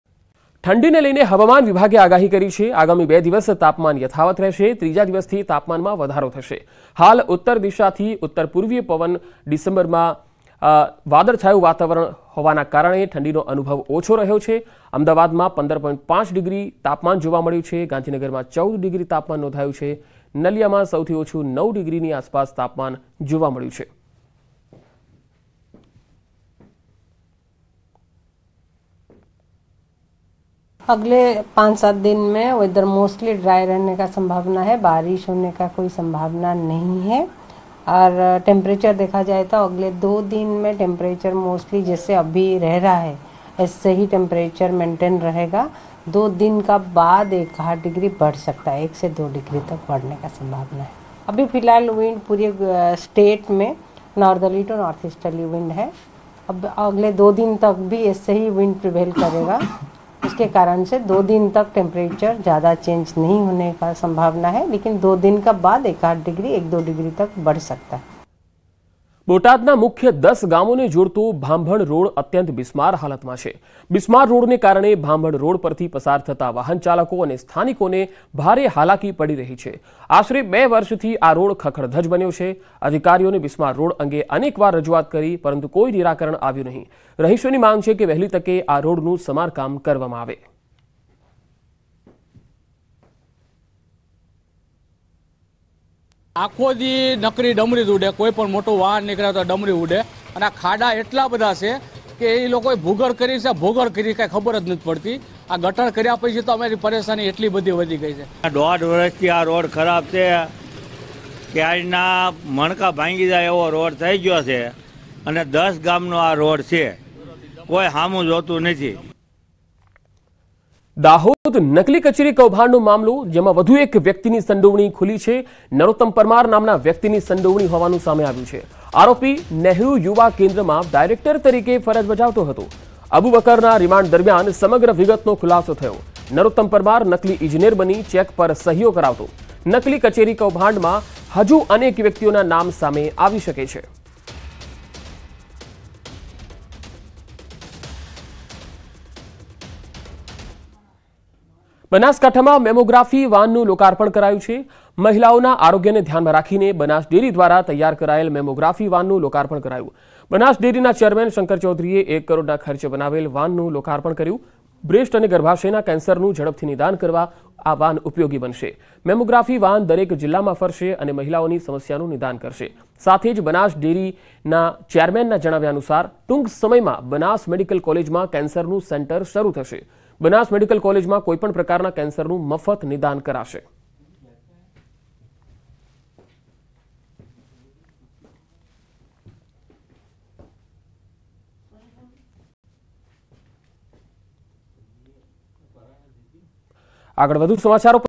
Gujarat Weather : ઠંડીને લઈને હવામાન વિભાગની આગાહી | Meteorological Department |Gujrati News |News18
Meteorological Department's forecast for cold weather. The temperature will remain unchanged for two days. There will be an increase in temperature from the third day. Presently north to north easterly winds.

ઠંડીને લઈને હવામાન વિભાગની આગાહી. બે દિવસ તાપમાન યથાવત રહેશે. ત્રીજા દિવસથી તાપમાનમાં વધારો હશે. હાલ ઉત્તર દિશાથી ઉત્તર પૂર્વીય પવનો. n18oc_gujarat